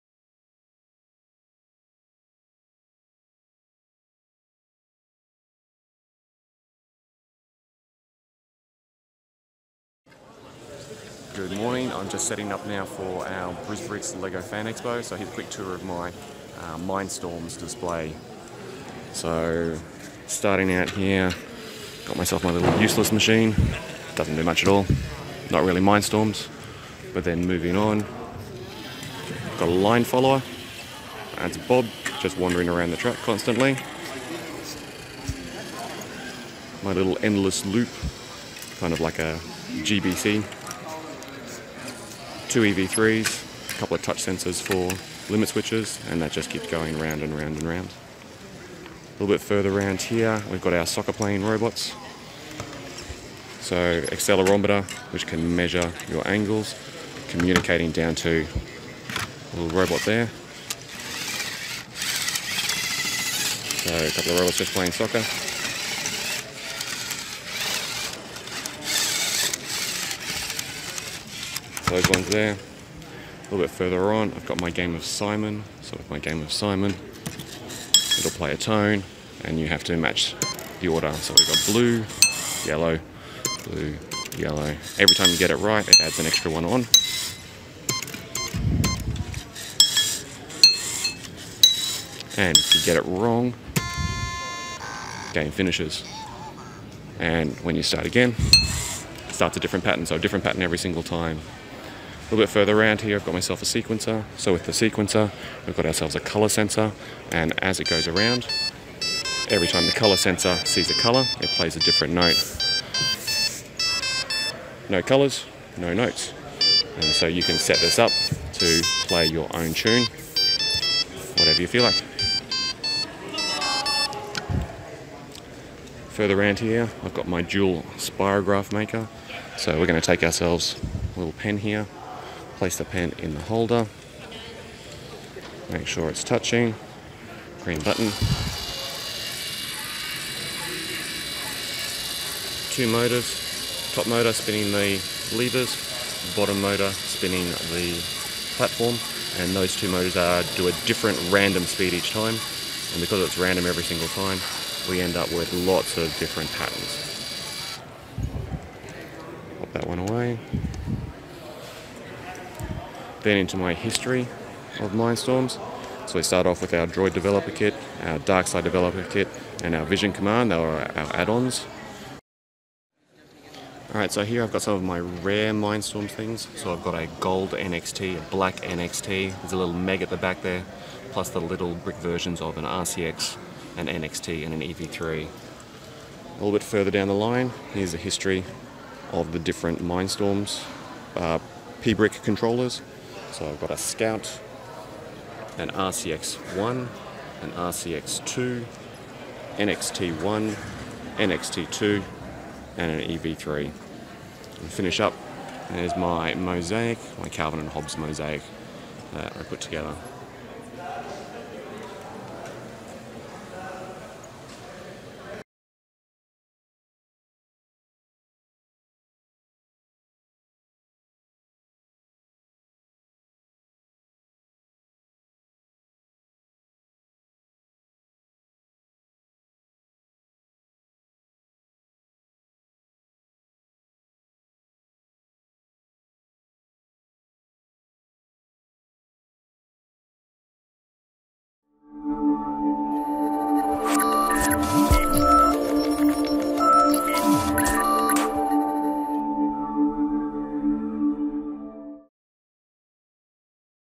Brisbricks 2018 MINDSTORMS Walkthrough
Here is a quick walk through of my display at the 2018 BrisBricks LEGo Fan Expo.  It won the 'Most Interactive Display' category for the weekend.

In the 'Interactive' part, there are 10 EV3 controllers, 20 Large Motors, 1 Medium Motor and 16 assorted Sensors.  The Spirograph went through 2000+ PostIt notes and about 20 pens! :)

My awesome volunteer helper Jacob and myself didn't stop talking for 3 days straight and there were times when our crowd were 3+ people deep.  Huge thanks to everyone that said hello, sorry I couldn't talk for longer.